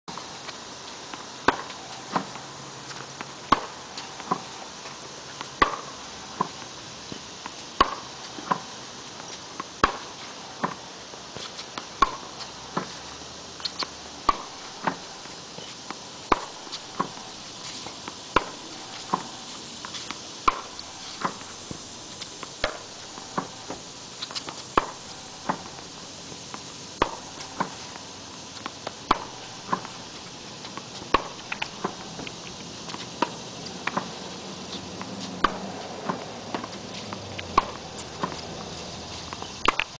Tennis Practice
hitting on the ball machine